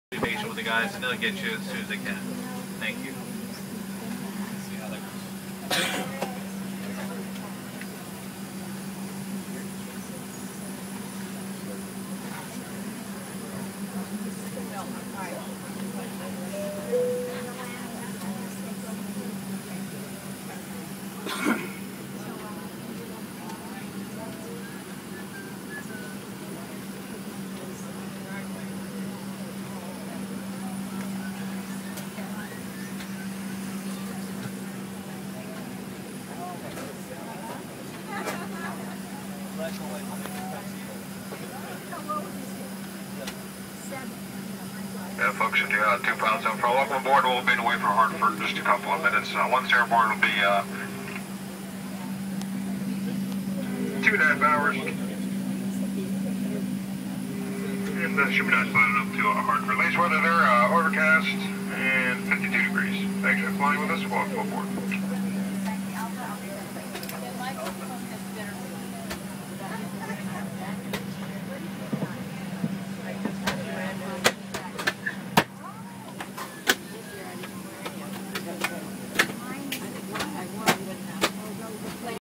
Service dogs on plane
Bradley and Louie prepare to head back home from their vacation.